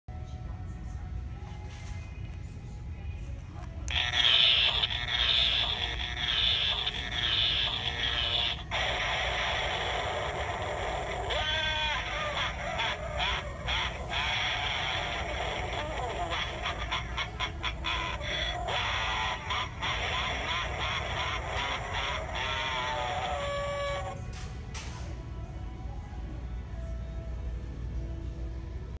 At Home Halloween 2017 Strobing Electric Reaper
Video taken at the Blaine, MN location.

Made by Sunstar Industries.